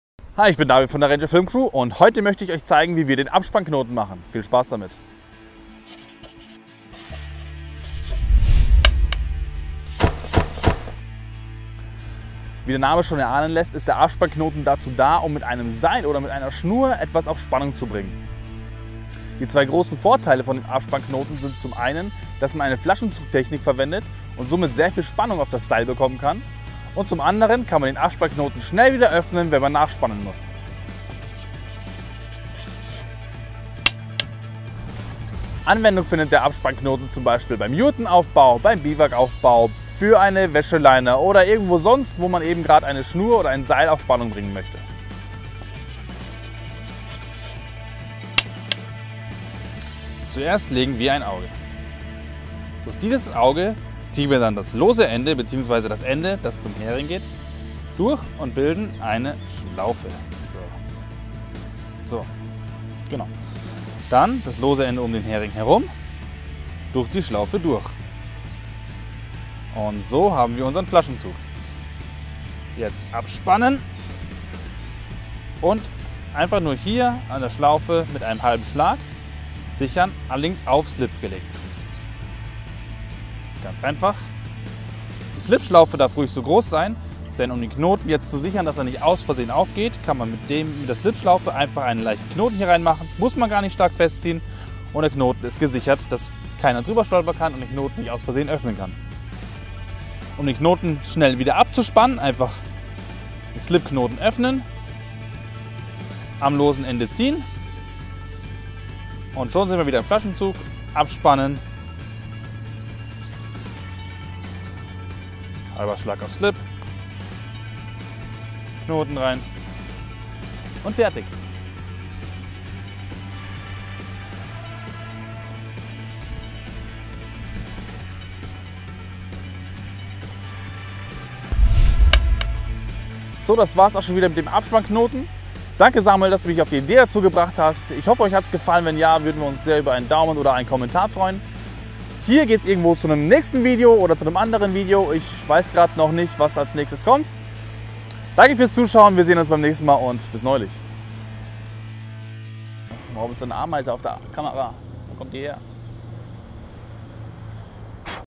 Knoten #6 Der Abspannknoten
Der Abspannknoten
Damit die Jurte richtig steht und die Wäscheleine hängt